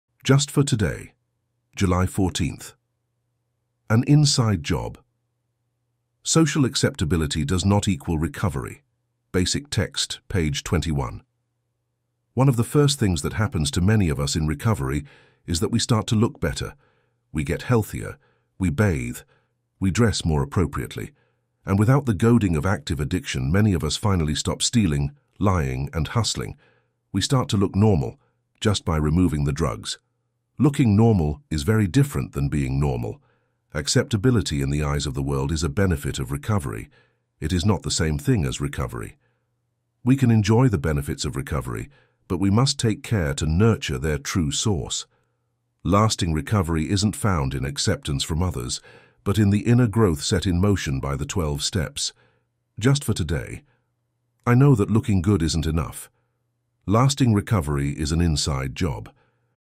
NA Just For Today - July 14 - An "inside job"
🌟 **Today's Meditation: An "inside job"** 🌟

"Social acceptability does not equal recovery"
Basic Text, page 21
One of the first things that happens to many of us in recovery is that we start to look
better. We get healthier; we bathe; we dress more appropriately. And without the
goading of active addiction, many of us finally stop stealing, lying, and hustling. We
start to look normal-just by removing the drugs.
Looking normal is very different than being normal. Acceptability in the eyes of the
world is a benefit of recovery; it is not the same thing as recovery. We can enjoy the
benefits of recovery, but we must take care to nurture their true source. Lasting
recovery isn't found in acceptance from others, but in the inner growth set in motion
by the Twelve Steps.
Just for today: I know that looking good isn't enough. Lasting recovery is an inside
job. 🛤️ The key to a successful and fulfilling recovery lies in focusing on a few core principles: sharing our experience, strength, and hope with others 💬, attending regular meetings 🗓️, and practicing the Twelve Steps in our daily lives 🏆. By doing so, we create a strong foundation that not only supports our personal growth but also helps newcomers find their path to recovery. 🙌

Sharing our stories and experiences with fellow addicts provides a powerful example and encourages others to stay committed to their journey. 👫 Practicing the Twelve Steps daily ensures we maintain a balanced and purposeful life, free from addiction. 🌈